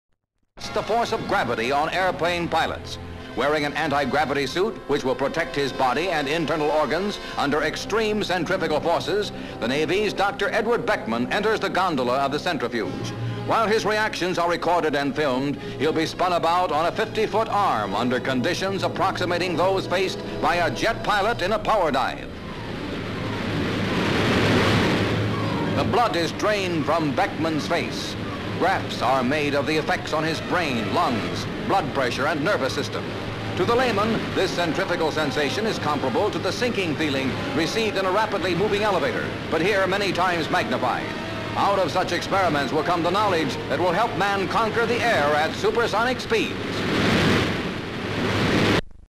Selected Originals - U.S. News - Flying Arm Centrifuge Test For Pilots (1952)
Selected originals (offcuts, selected scenes, out-takes, rushes) for "U.S. News - Flying Arm Centrifuge Test For Pilots" 52/53.

Material for newsreel story - American voiceover

Johnsville, Pennsylvania.

SV: Dr. Edward Beckman about to receive injection, prior to making test in Centrifuge. SV. Beckman being fitted with antigravity suit. SCU. Beckman, walks away towards gondola of centrifuge, he is about to test his body under high G-forces. LV. Beckman descending steps and entering gondola. SV. Beckman seated in gondola. SCU. Beckman, Assistants replacing door on Gondola. LV. Towards and pan, centrifuge begins to revolve. CU. Beckman seated in gondola. SV. Graphs recording effect on Beckman's brain etc., SV. Centrifuge revolving. SV. Controller. SV. Centrifuge whipping past camera. CU. Beckman seated in gondola CU. Gondola whipping past camera, into LV. Beckman seated in gondola.
 FILM ID:14.55